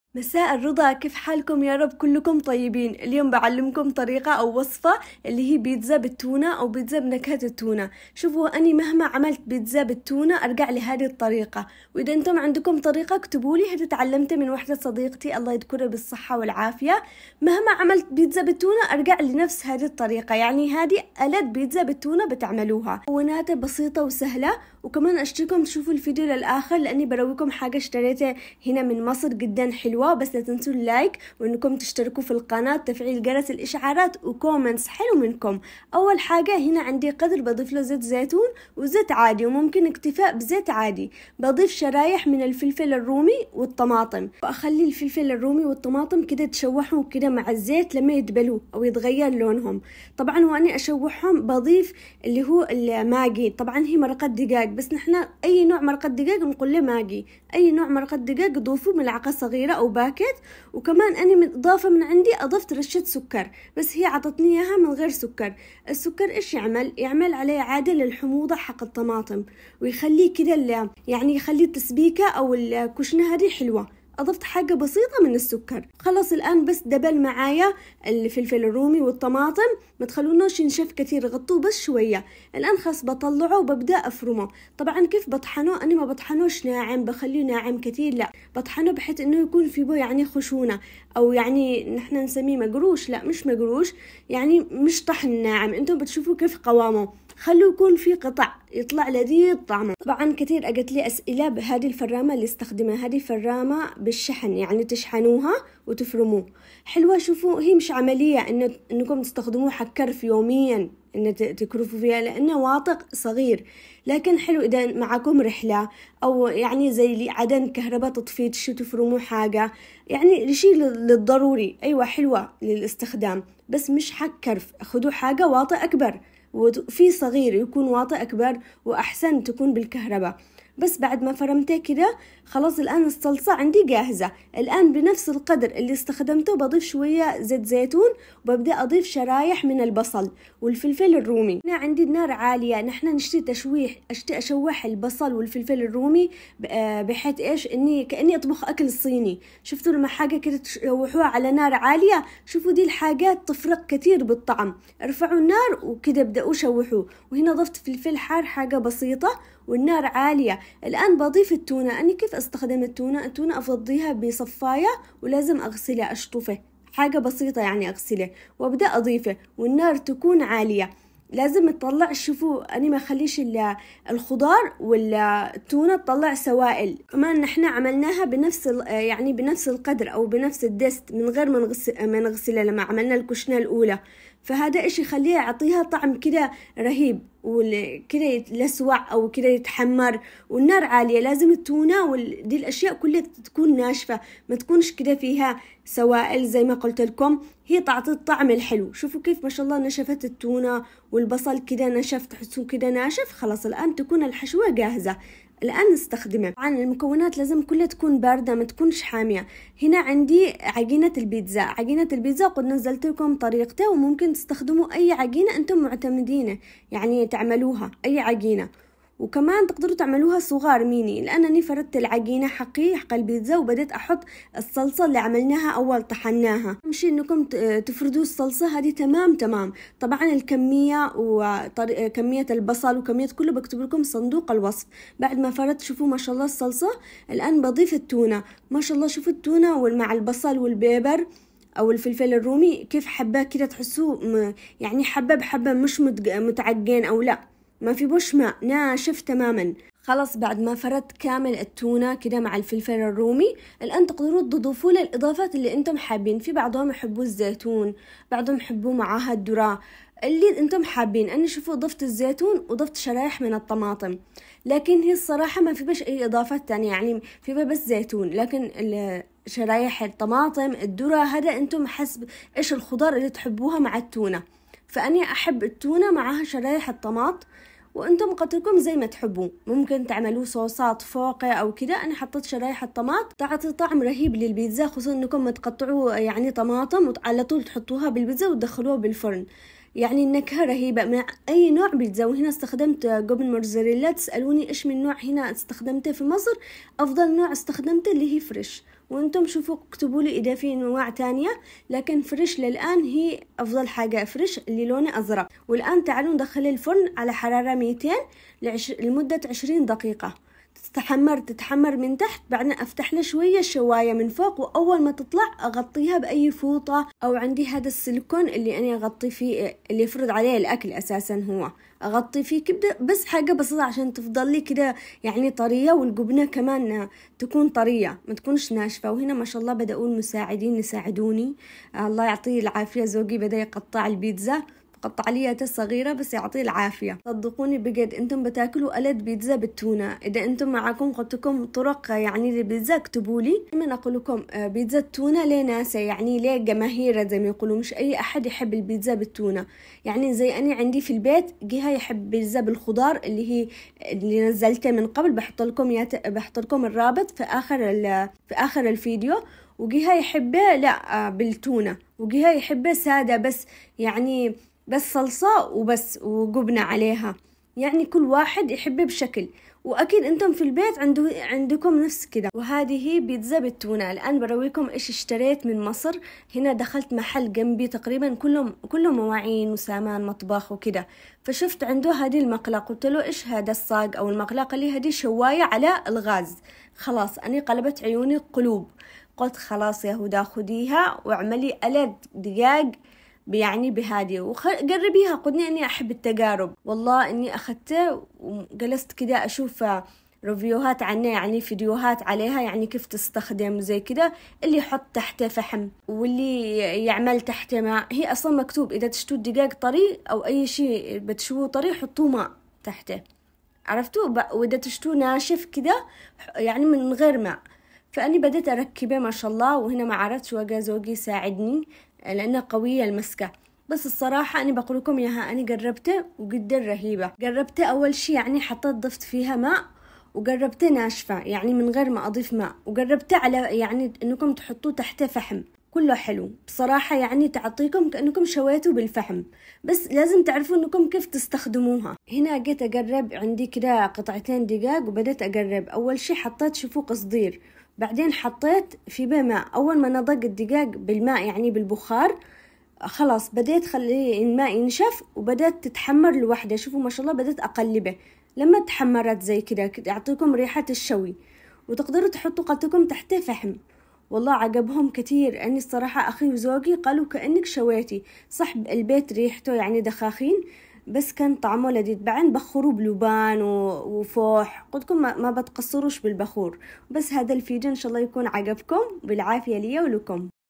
عندك تونة ومكعب مرقة دجاج؟ حضري الذ بيتزا
وصفات باتونة
بيتزا باتونة
مكونات بيتزا التونة
2 حبة فلفل رومي
2 حبة طماطم
1 حبة كبيرة بصل
مكعب مرقة دجاج
علبة تونة
ملعقة صغيرة سكر
2 ملاعق زيت زيتون
2 ملاعق زيت درة
جبن مورزريلا
زيتون
شرائح طماطم
ممكن اضافة رشة ملح حسب الرغبة مع مراعات مكعب المرقة مالح و الزيتون